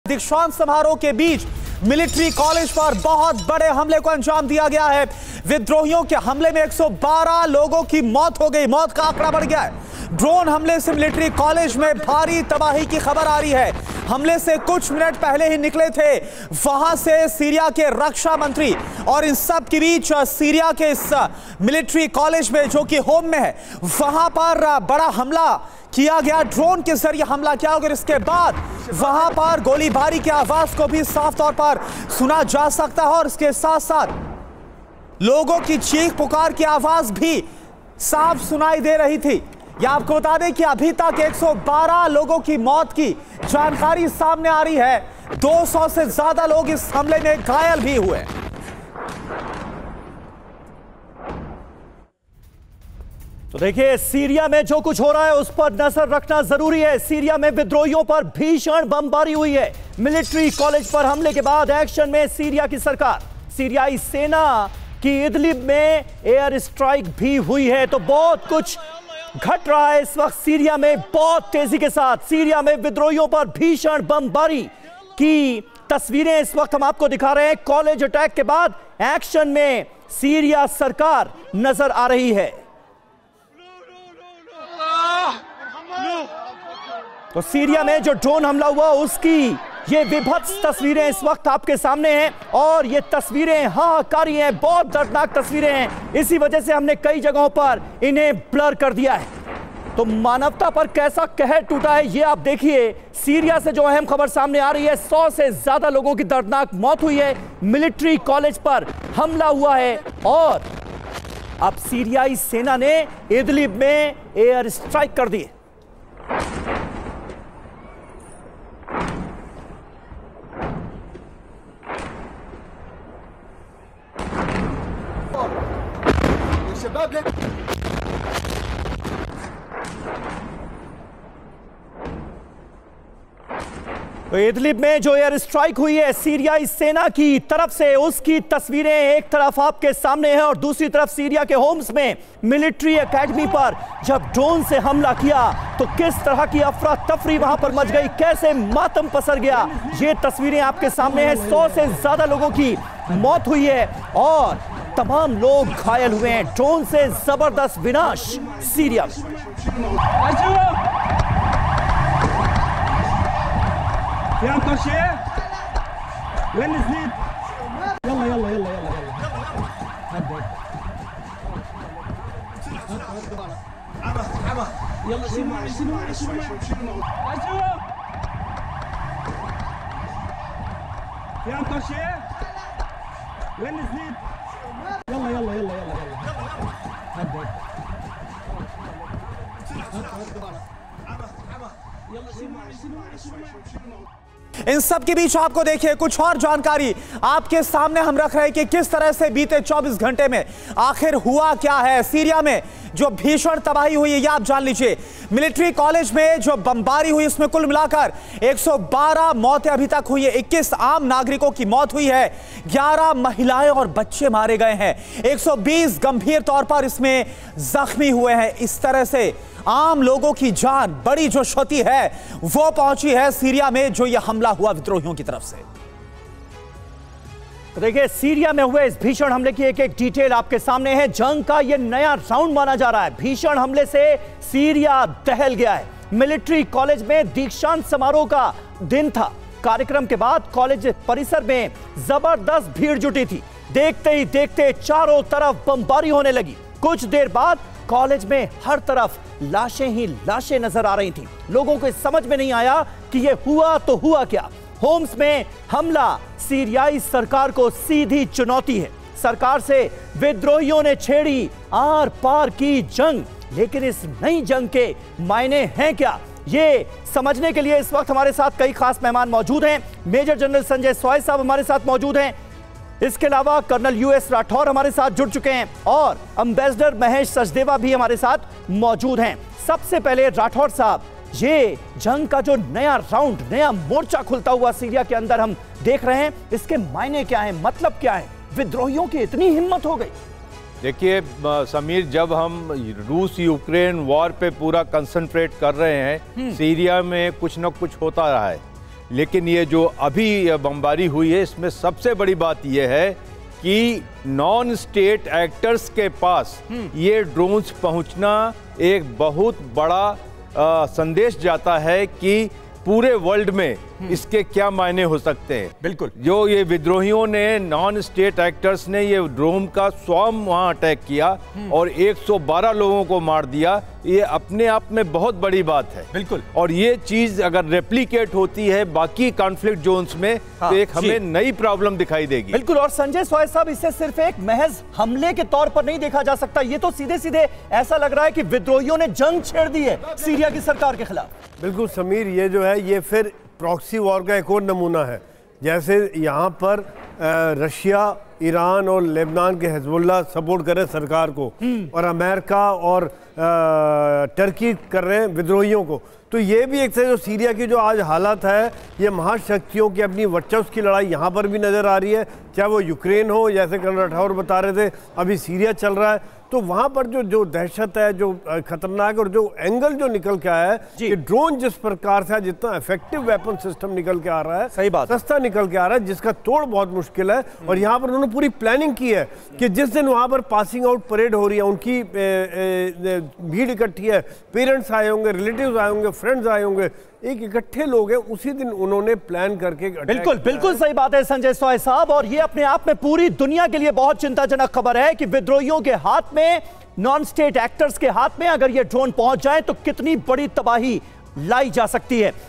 Syria में भीषण जंग जैसे हालात, दीक्षांत समारोह के बीच Military College पर हमला
सीरिया में भीषण जंग जैसे हालात, दीक्षांत समारोह के बीच मिलिट्री कॉलेज पर हमला, विद्रोहियों के हमले में 112 लोगों की मौत, ड्रोन हमले से मिलिट्री कॉलेज में भारी तबाही, हमले से कुछ मिनट पहले ही निकले थे रक्षा मंत्री.
Fierce war-like situation in Syria, attack on military college during convocation ceremony, 112 people killed in rebel attack, huge devastation in military college due to drone attack, Defense Minister had left just minutes before the attack.